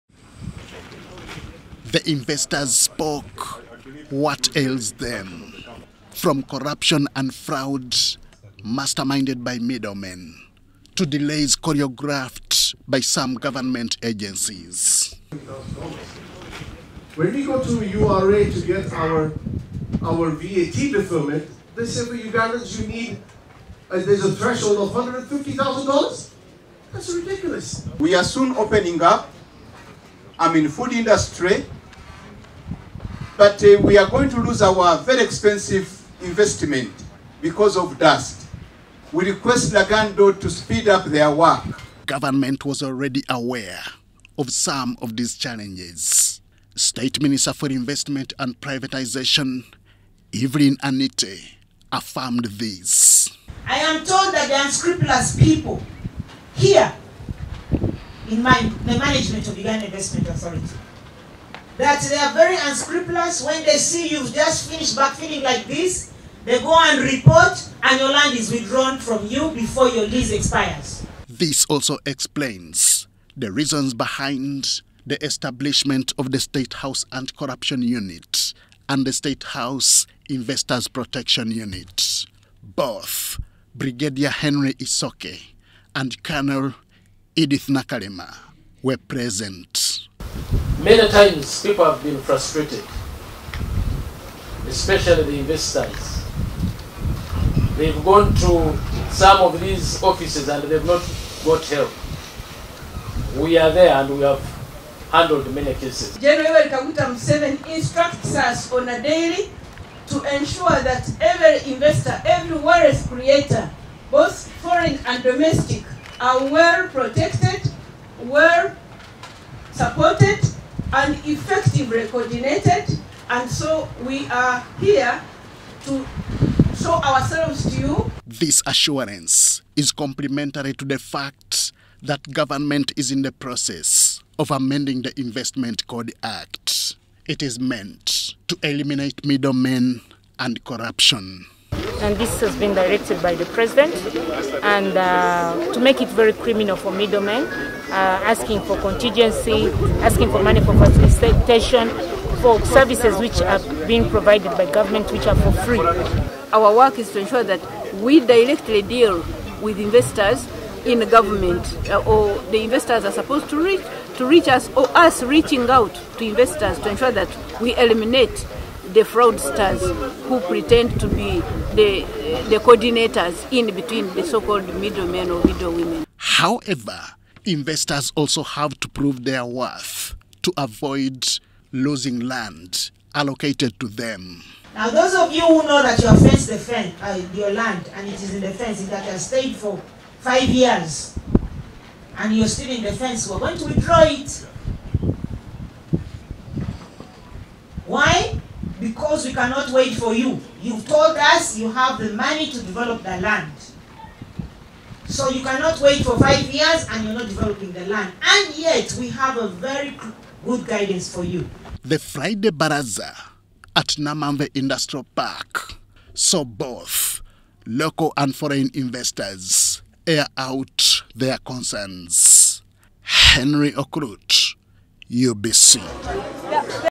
An eye on investment - Government to withdraw undeveloped land from investors in Namanve
Government is threatening to withdraw Land from investors at Namanve Industrial Park who have not developed their portions for the last five years.

State Minister for investment and Privatization Evelyn Anite says such delays deny the country avenues to avail employment opportunities to jobless youth.
This she said during an engagement with local and foreign investors at Namanve Industrial Park where she revealed government plans to amend the investment code Act.